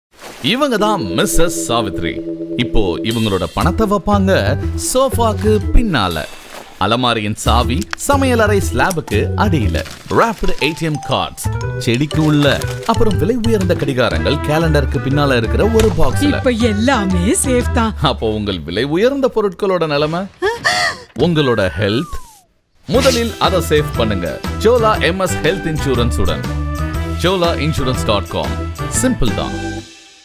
Chola MS | Health Insurance
You checked the door lock thrice, and your health insurance 0 times. Not fair.
Insure your most important valuable (your health!) and avail a range of benefits and cashless services from 9500+ hospitals across the country!